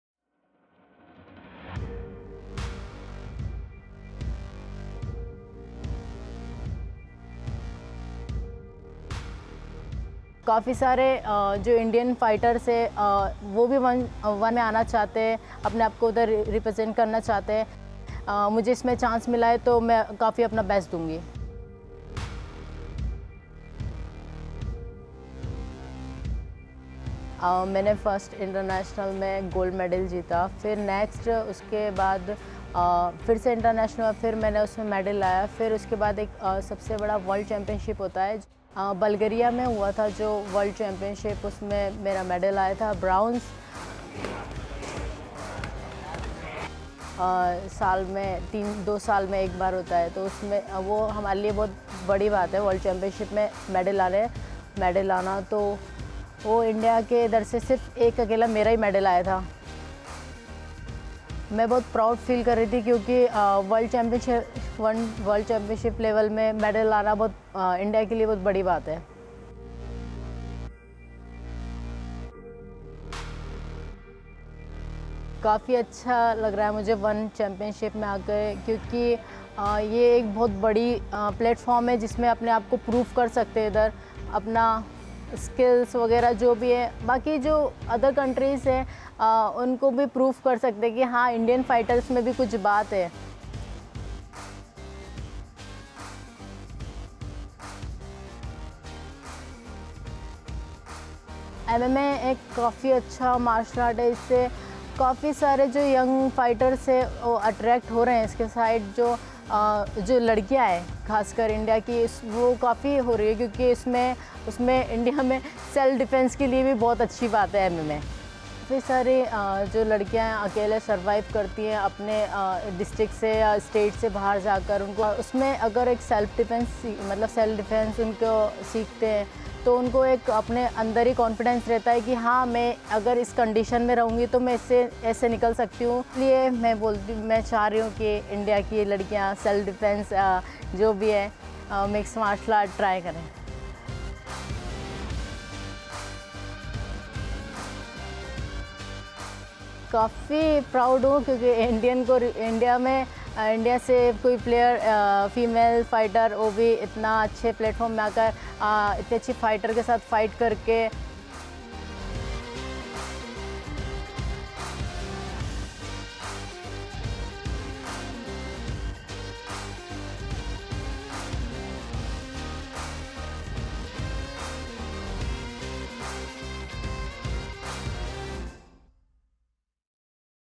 Asha Roka Makes Her Mark On The Global Stage | ONE Feature
Asha Roka is one of the most exciting young talents ever to arrive in ONE Championship. The Indian national boxing champion will enter the ONE ring with a perfect 4-0 record and 100-percent finish rate as a professional mixed martial artist. But Roka’s personal mission transcends any the accolades, and she hopes that her success on the international stage will inspire India's billion-plus population to chase their own dreams. She welcomes two-sport ONE World Champion Stamp Fairtex to the world of mixed martial arts at ONE: DREAMS OF GOLD in Bangkok.